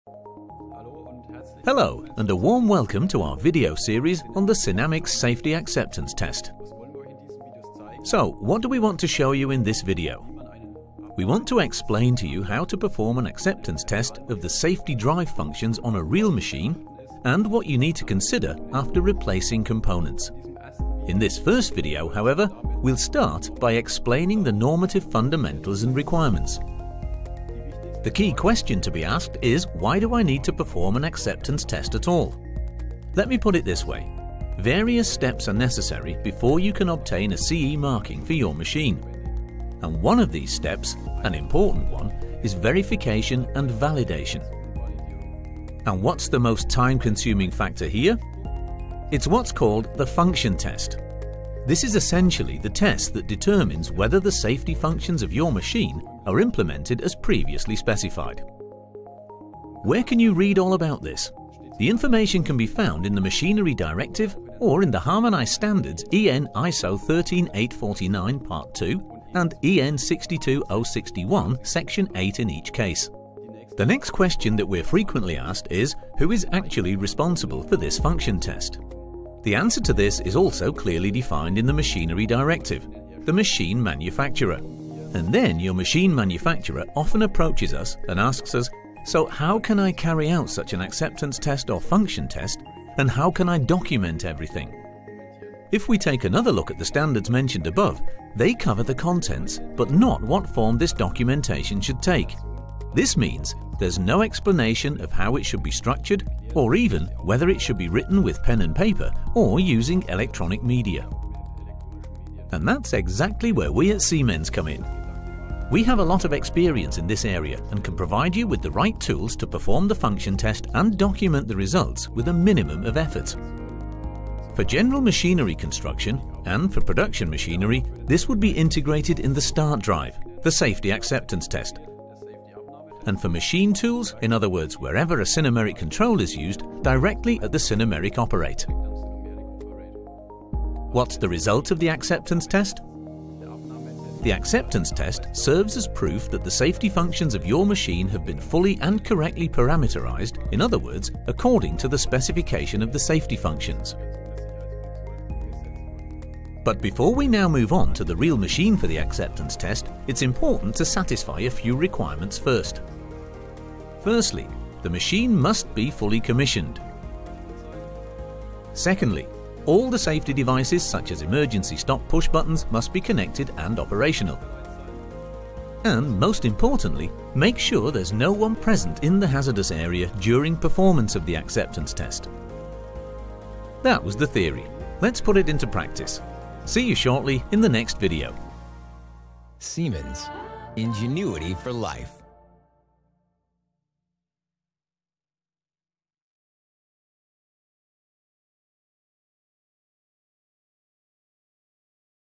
SINAMICS Safety acceptance test - #1 normative background
The way to a safe machine - In this video series we show why an acceptance test of the safety functions is required and how it can be carried out very easily with SINAMICS Startdrive Advanced.
This video explains the normative background to the acceptance test of the safety functions. Why do you need an acceptance test? Who is responsible for it? What has to be considered?